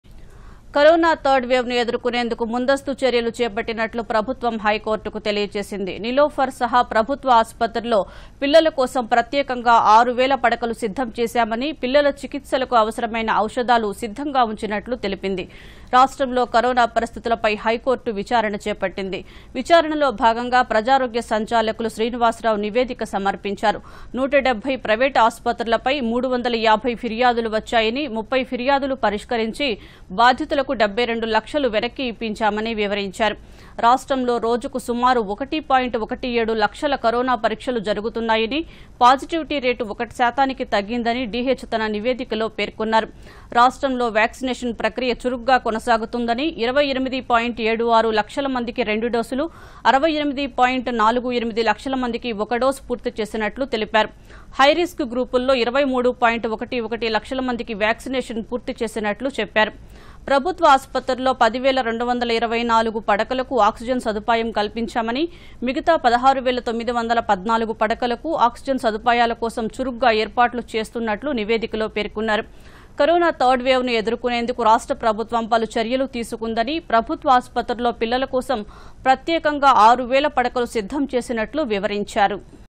HC: Director Public Health Srinivasa Rao-30 complaints resolved & Rs72 lakh returned to victims
కొరోనా థర్డ్ వేవ్ ను ఎదుర్కొనేందుకు ముందస్తు చర్యలు చేపట్టినట్లు ప్రభుత్వం.. హైకోర్టుకు తెలియజేసింది. నీలోఫర్‌ సహా ప్రభుత్వ ఆస్పత్రుల్లో పిల్లల కోసం ప్రత్యేకంగా 6వేల పడకలు సిద్ధం చేశామని,  పిల్లల చికిత్సలకు అవసరమైన ఔషధాలు సిద్ధంగా ఉంచినట్లు తెలిపింది. రాష్ట్రంలో కొరోనా పరిస్థితులపై హైకోర్టు విచారణ చేపట్టింది. విచారణలో భాగంగా ప్రజారోగ్య సంచాలకులు శ్రీనివాసరావు నివేదిక సమర్పించారు. 170 ప్రైవేటు ఆస్పత్రులపై 350 ఫిర్యాదులు వచ్చాయని.. 30 ఫిర్యాదులు పరిష్కరించి బాధితులకు 72 లక్షలు వెనక్కి ఇప్పించామని వివరించారు. రాష్ట్రంలో రోజుకు సరాసరి 1.17లక్షల కరోనా పరీక్షలు జరుగుతున్నాయని, పాజిటివిటీ రేటు 1 శాతానికి తగ్గిందని డీహెచ్‌ తన నివేదికలో పేర్కొన్నారు. రాష్ట్రంలో వ్యాక్సినేషన్‌ ప్రక్రియ చురుగ్గా కొనసాగుతోందని,  28.76లక్షల మందికి రెండు డోసులు, 68.48లక్షల మందికి ఒక డోసు పూర్తి చేసినట్లు తెలిపారు. హైరిస్కు గ్రూపుల్లో 23.11 లక్షల మందికి వ్యాక్సినేషన్‌ పూర్తి చేసినట్లు చెప్పారు.  ప్రభుత్వ ఆస్పత్రుల్లో 10,224 పడకలకు ఆక్సిజన్‌ సదుపాయం కల్పించామని,  మిగతా 16,914 పడకలకు ఆక్సిజన్‌ సదుపాయాల కోసం చురుగ్గా ఏర్పాట్లు చేస్తున్నట్లు నివేదికలో పేర్కొన్నారు. కొరోనా థర్డ్ వేవ్ ను ఎదుర్కొనేందుకు రాష్ట్ర  ప్రభుత్వం పలు చర్యలు తీసుకుందని, ప్రభుత్వ ఆస్పత్రుల్లో పిల్లల కోసం ప్రత్యేకంగా 6వేల పడకలు సిద్ధం చేసినట్లు వివరించారు.